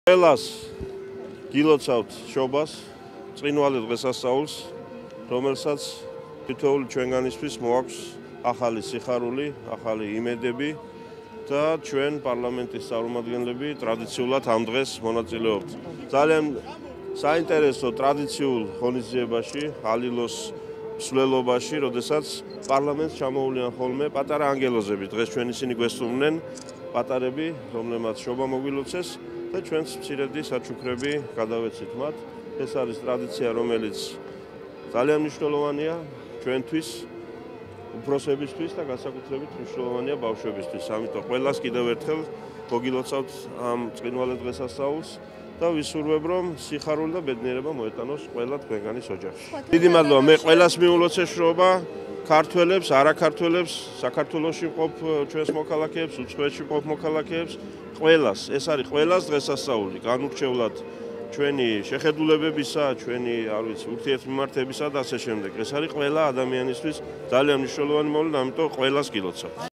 დავით უსუფაშვილის საშობაო მილოცვა
შობის დღესასწაულთან დაკავშირებით, საქართველოს პარლამენტის თავმჯდომარე დავით უსუფაშვილი და პარლამენტის წევრები ტრადიციული საშობაო ალილოს მსვლელობის მონაწილეებს თბილისში, პარლამენტის სასახლის წინ შეეგებნენ. დავით უსუფაშვილმა და დეპუტატებმა მსვლელობის მონაწილეებს ტკბილეულით სავსე კალათები გადასცეს.

„ყველას გილოცავთ შობას, ბრწყინვალე დღესასწაულს, რომელსაც თითოეული ჩვენგანისთვის მოაქვს ახალი სიხარული, ახალი იმედები და ჩვენ, პარლამენტის წარმომადგენლები, ტრადიციულად, ამ დღეს, ვმონაწილეობთ ძალიან საინტერესო, ტრადიციულ ღონისძიებაში, ალილოს მსვლელობაში, როდესაც პარლამენტს ჩამოუვლიან ხოლმე პატარა ანგელოზები. დღეს ჩვენ ისინი გვესტუმრნენ, პატარები, რომლებმაც შობა მოგვილოცეს და ჩვენც მცირედი საჩუქრები გადავეცით მათ. ეს არის ტრადიცია, რომელიც ძალიან მნიშვნელოვანია ჩვენთვის, უფროსებისთვის და განსაკუთრებით მნიშვნელოვანია ბავშვებისთვის, ამიტომ ყველას კიდევ ერთხელ მოგილოცავთ ამ ბრწყინვალე დღესასწაულს და ვისურვებ, რომ სიხარული და ბედნიერება მოეტანოს ყველა თქვენგანის ოჯახში“,- განაცხადა პარლამენტის თავმჯდომარემ.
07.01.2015